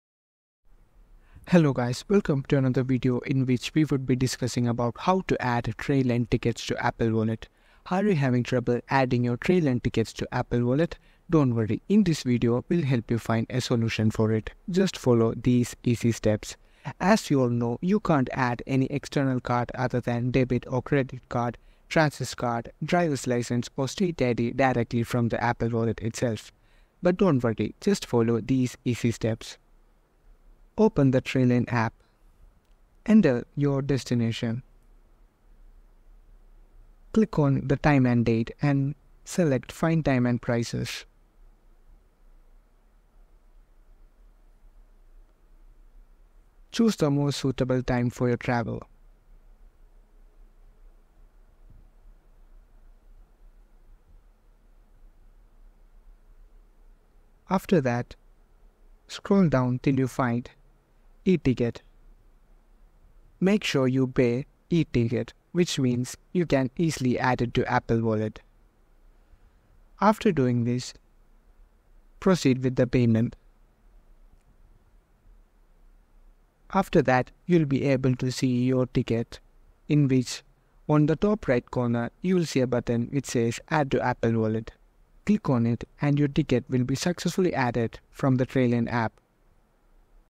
How To Add Trainline Tickets To Apple Wallet
In this tutorial video, learn how to easily add Trainline tickets to your Apple Wallet for convenient access and seamless travel experience. Follow the step-by-step guide to ensure your tickets are readily available on your iPhone whenever you need them.